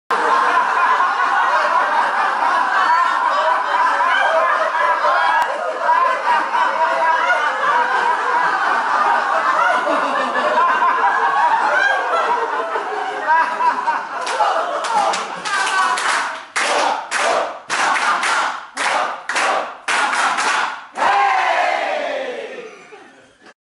Atelier du rire à la Réunion
Extrait d'un atelier de Yoga du Rire sur l'île de la Réunion au Domaine des Eucalyptus aux Colimaçons